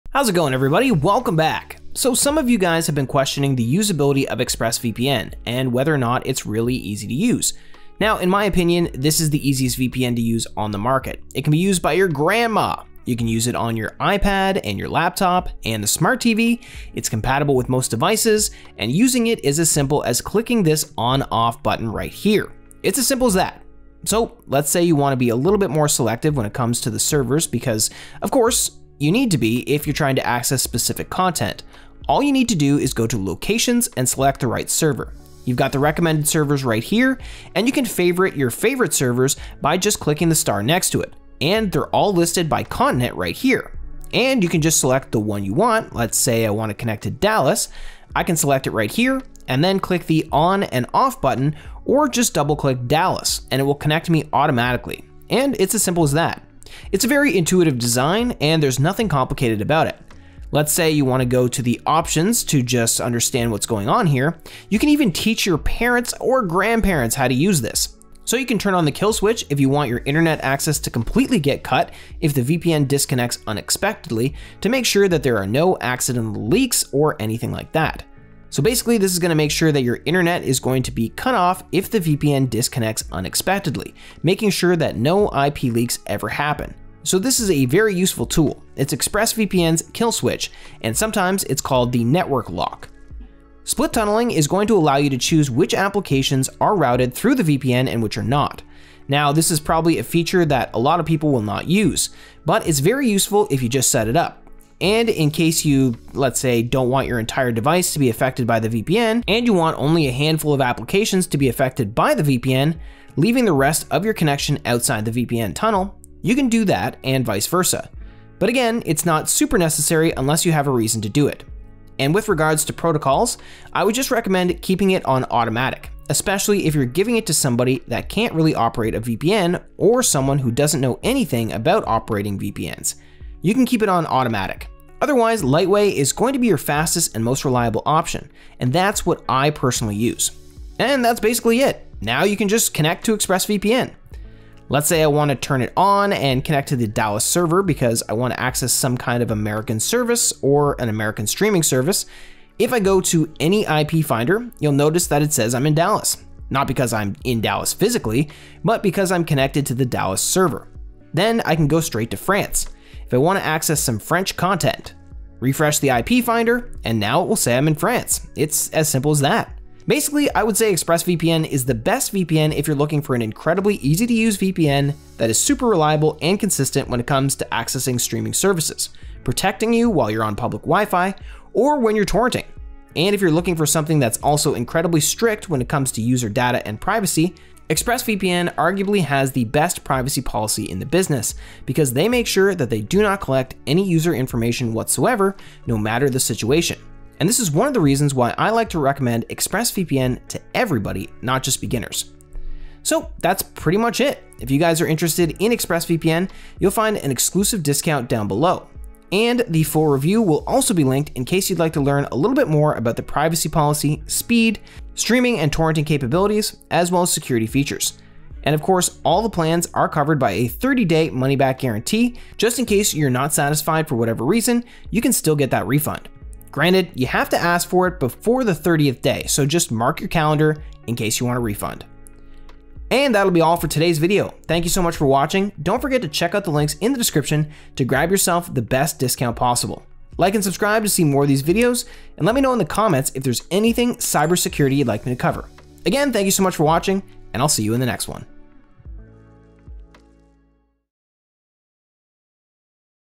Is ExpressVPN User-Friendly? My Personal Experience with Express VPN
🌐Is ExpressVPN User-Friendly? My Personal Experience with ExpressVPN🌐

ExpressVPN is hailed as the epitome of simplicity in the VPN market. Its user-friendly interface makes it accessible to users of all ages and tech-savviness levels. With just a click of a button, you can toggle the VPN on and off across multiple devices, from your laptop to your Smart TV.

The server selection process is equally straightforward, allowing users to easily navigate through locations and favorite preferred servers. Plus, features like the kill switch and split tunneling are presented in an intuitive manner, making it easy for even the least tech-savvy individuals to understand and utilize.

ExpressVPN offers a robust set of features aimed at enhancing user experience and security. The kill switch, also known as the network lock, ensures that your internet access is cut off in the event of unexpected VPN disconnection, preventing any potential IP leaks. Split tunneling provides users with the flexibility to choose which applications are routed through the VPN, offering a customizable approach to online privacy.

When it comes to protocols, the automatic setting caters to those unfamiliar with VPN operation, while Lightway stands out as the fastest and most reliable option for experienced users. With ExpressVPN, accessing geo-restricted content and securing your online activities, whether it's streaming or torrenting, is a breeze.

In conclusion, ExpressVPN stands out as the premier choice for users seeking an easy-to-use, reliable VPN solution. Its intuitive design, coupled with robust security features and strict privacy policies, makes it a top contender in the market. If you're looking for a VPN that prioritizes user privacy, seamlessly unlocks streaming services, and provides consistent performance, ExpressVPN is the way to go.

Don't miss out on the exclusive discount available in the description, and be sure to check out the full review for a detailed breakdown of its capabilities. Remember, all plans are backed by a 30-day money-back guarantee, ensuring risk-free exploration.

Hope you enjoyed my Is ExpressVPN User-Friendly? My Personal Experience with Express VPN Video.